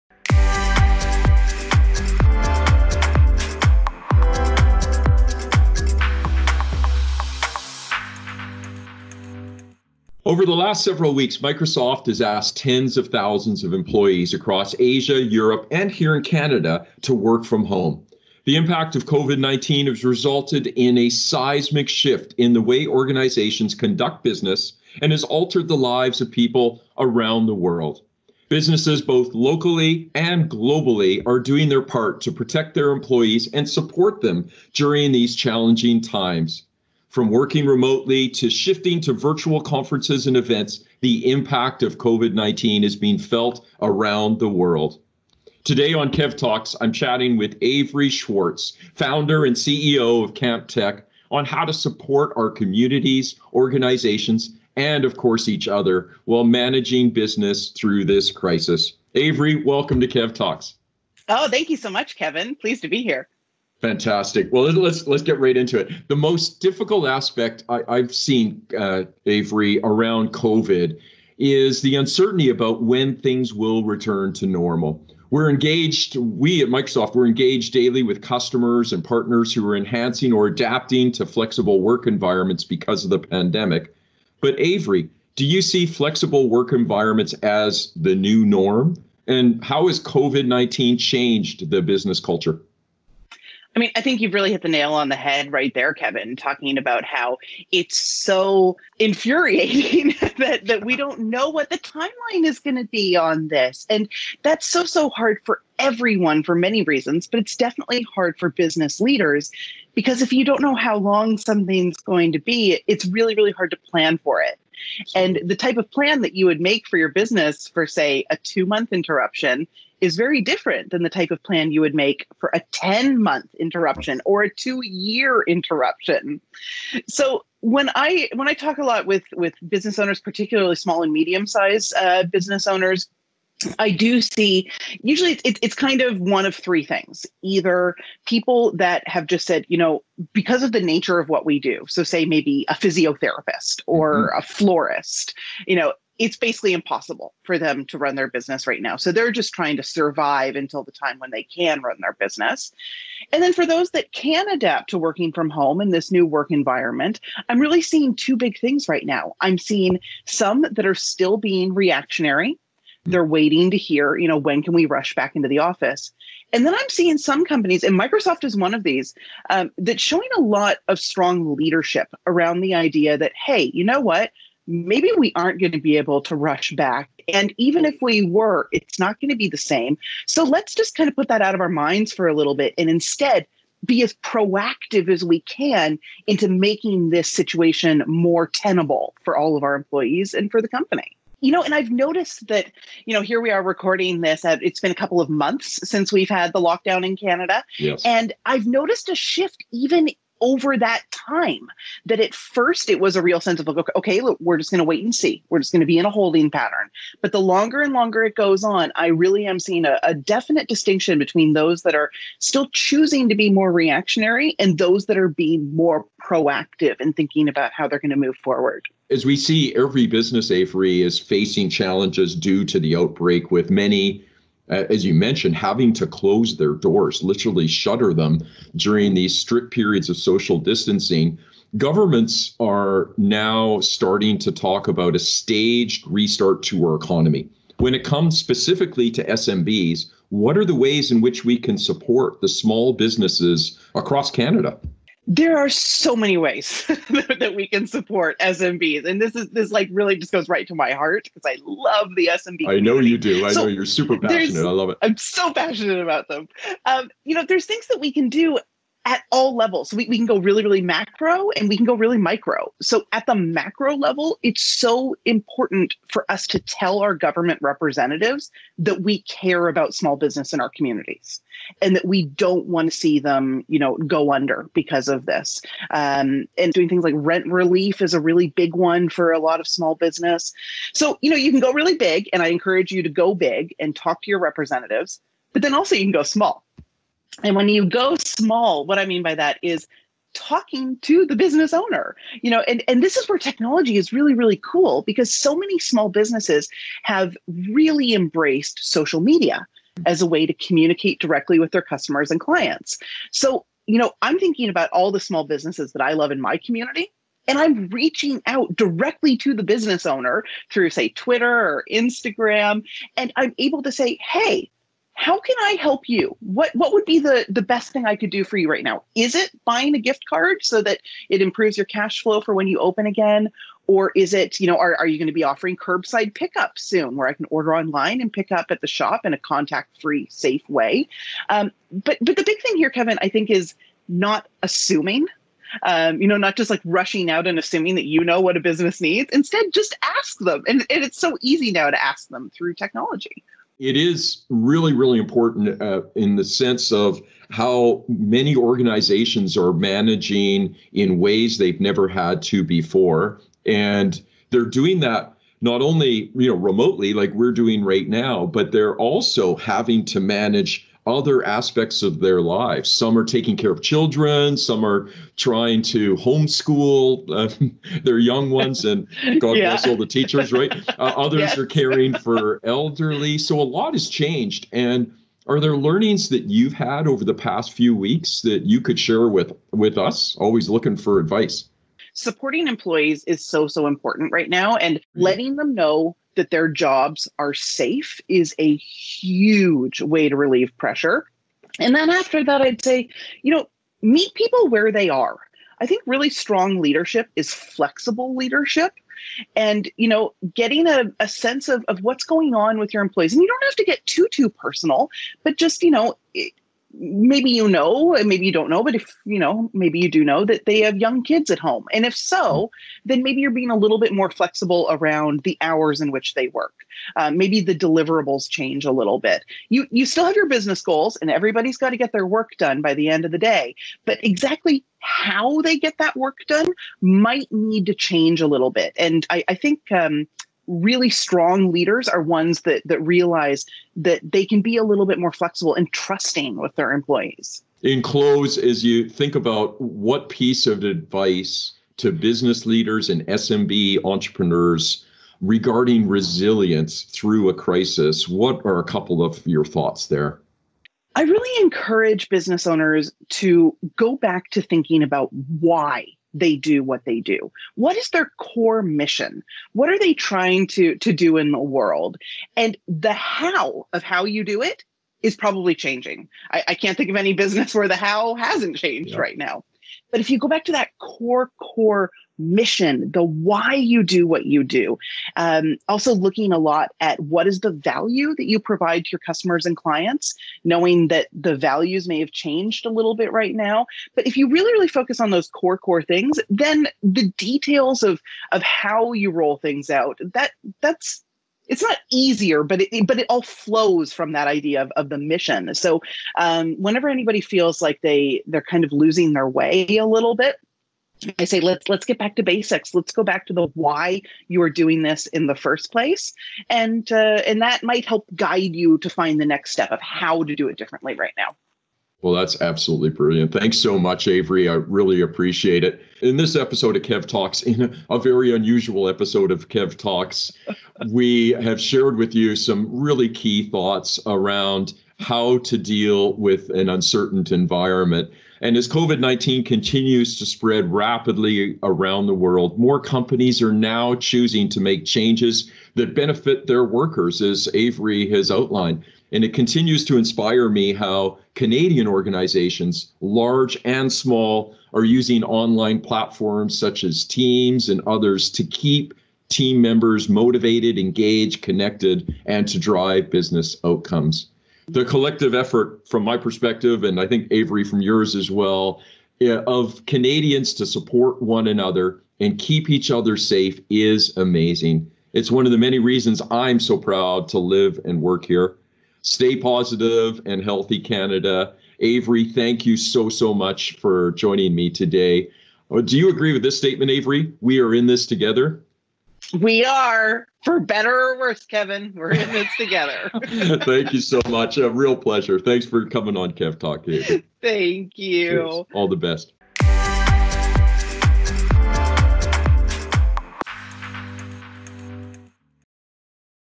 KEVtalks episode 11 with Avery Swartz
Covid-19 has created a seismic shift in the work environment for most businesses. In the latest episode of KEVtalks, I had a great virtual discussion with Avery Swartz, Founder & CEO of Camp Tech on how a flexible work environment is more important than ever, and the ways we can support Canadian SMBs impacted during this challenging time. Stay positive and healthy Canada, and remember - we are in this together!